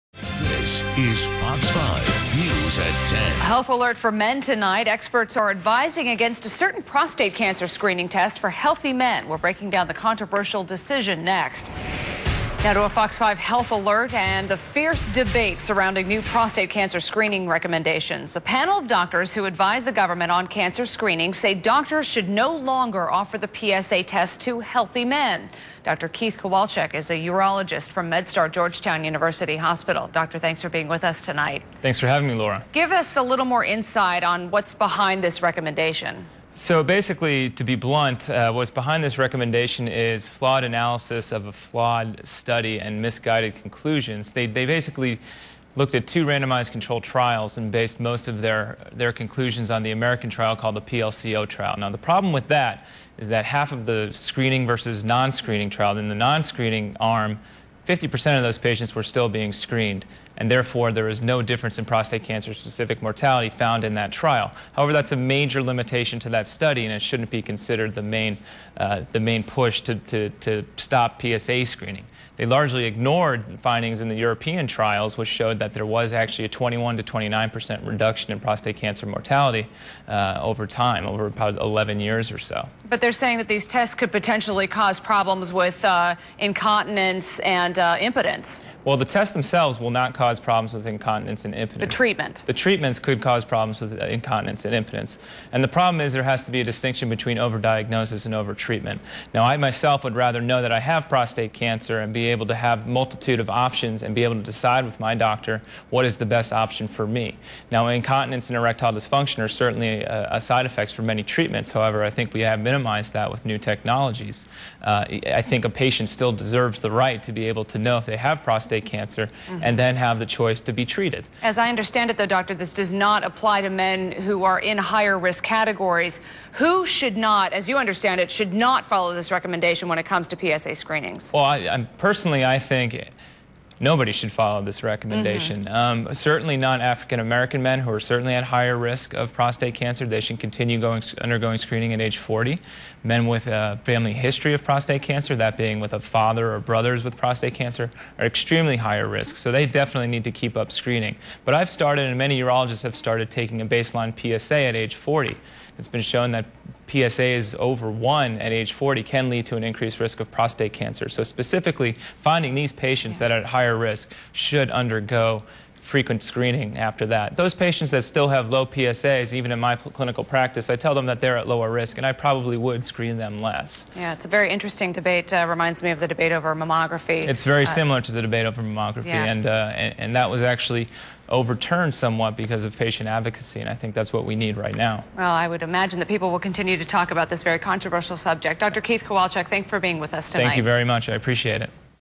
Dr. Keith Kowalczyk Discusses Prostate Cancer Screening (PSA) (Fox 5)
Dr. Kowalczyk is interviewed by Fox 5 in May 2012 about recommended guidelines on Prostate Cancer Screenings and the Prostate-Specific Antigen (PSA) Test.